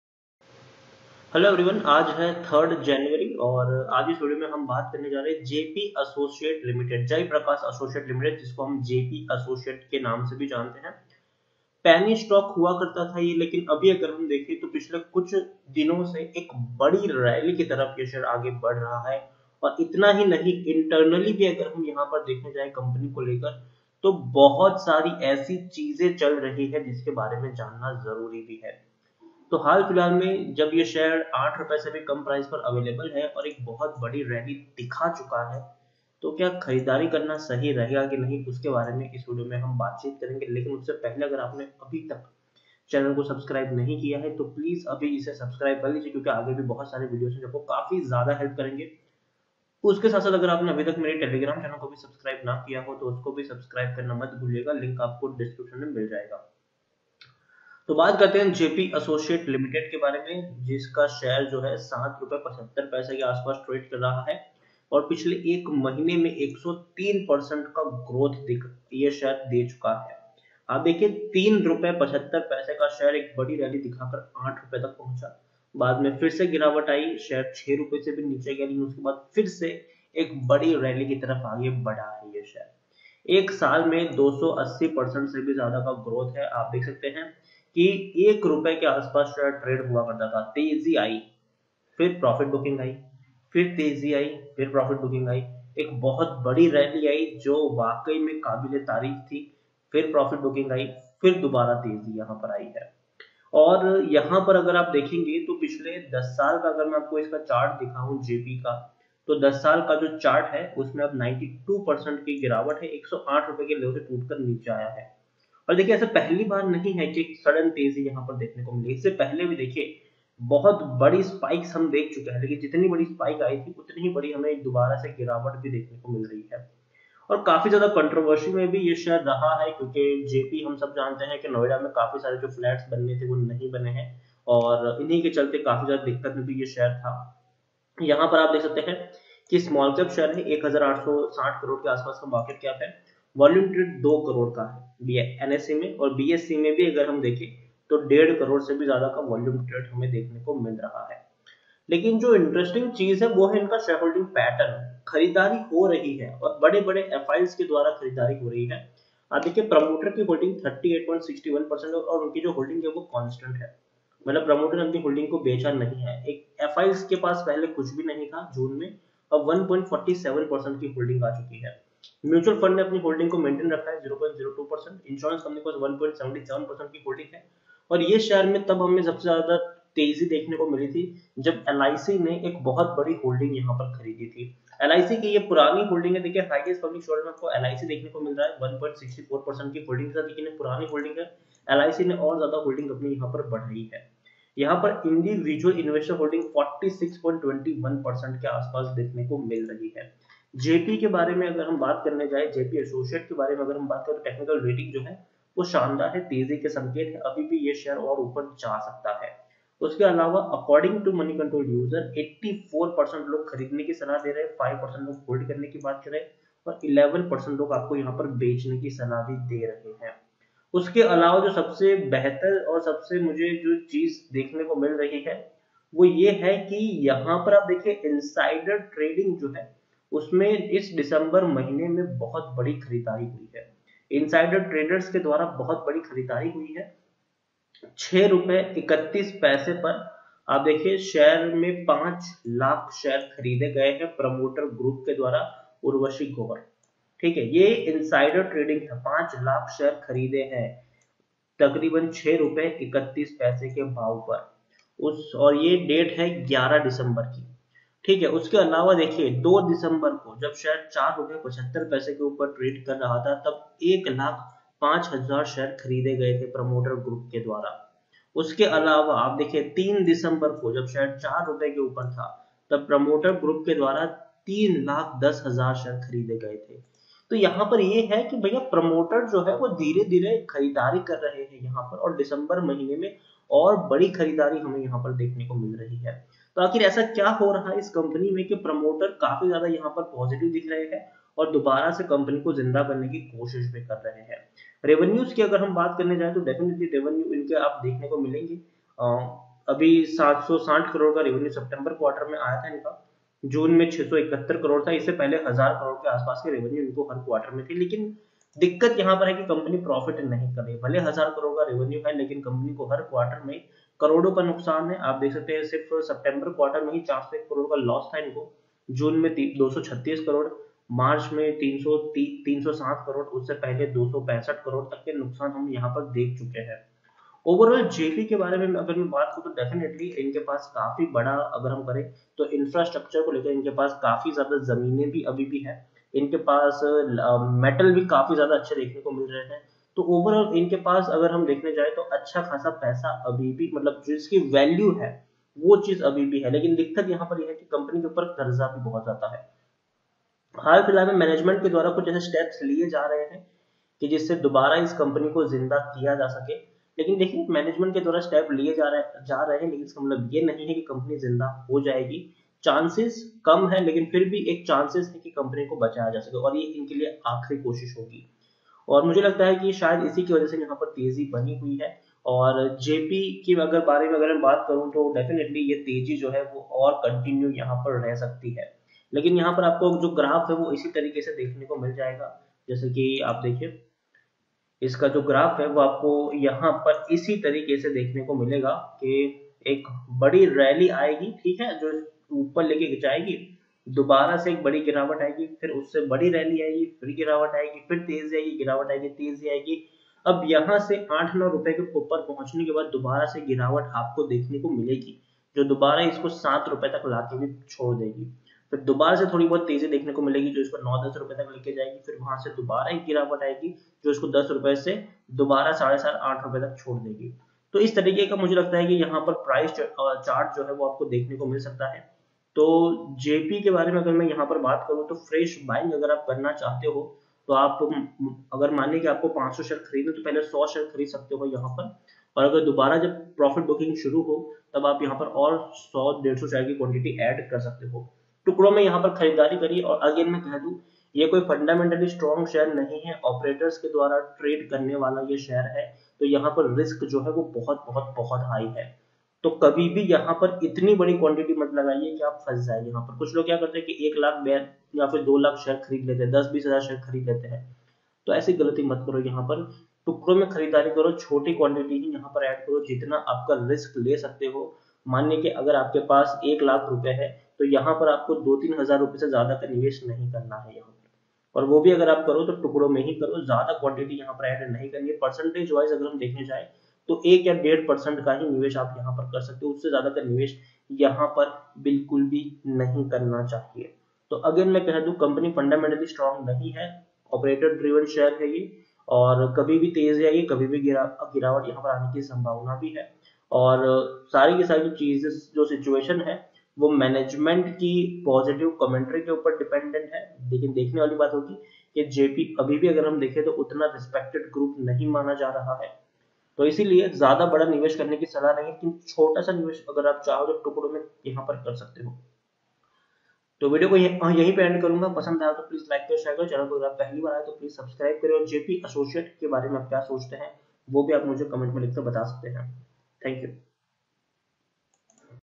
JP Associates Share Latest News | 8 रुपये का शेयर, 8 गुना मुनाफा, मौका या धोखा? | Penny Stock
JP Associates Latest News
JP Associates Share Update